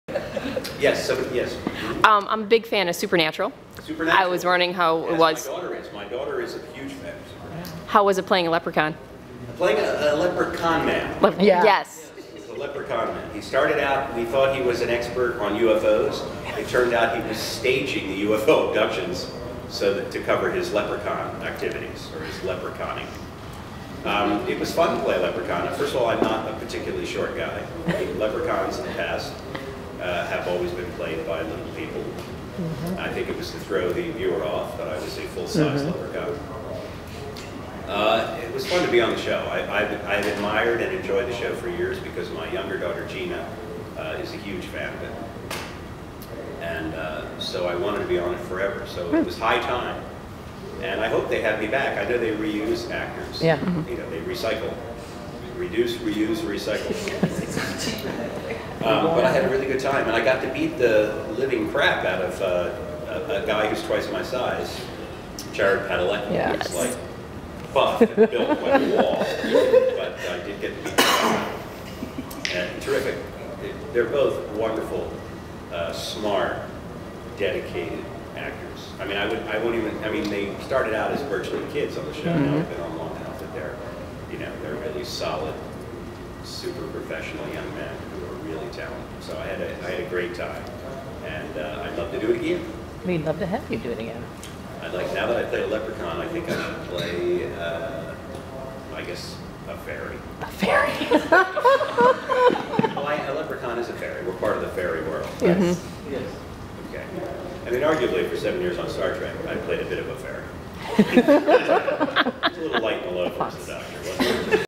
Robert Picardo talks Supernatural.mp4
Robert Picardo talking about filming Supernatural at the Cyphan 2 convention in Chicago, July 2011